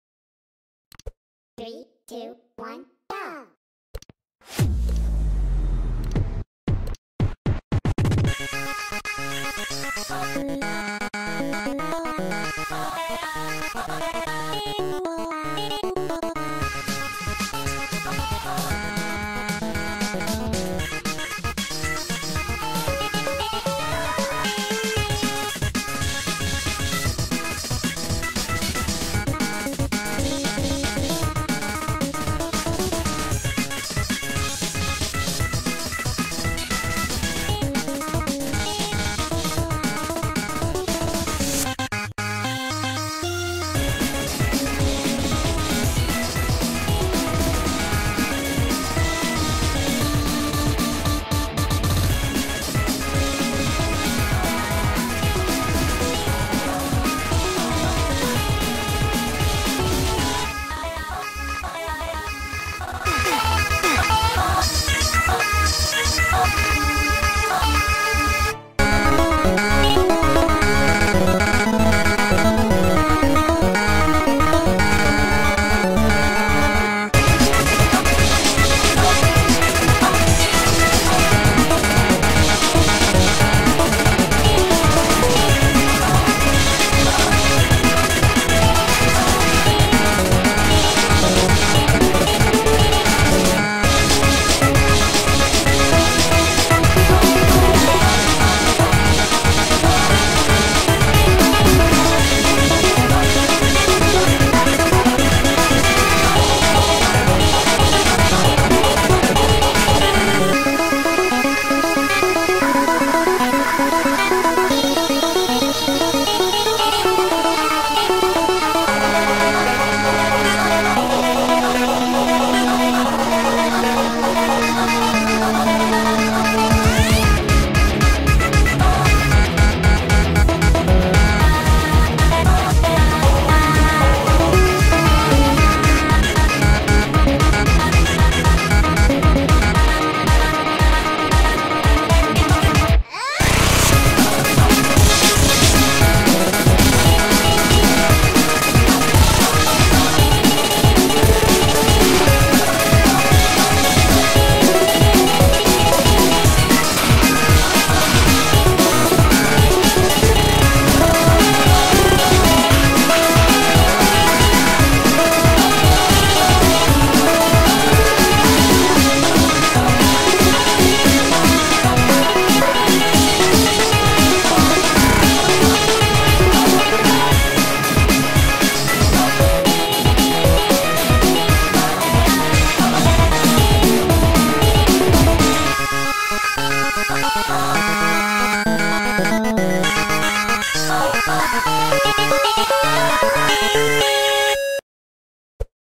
The exurggation fc (hi aquilo)
is literally me and aquilo singing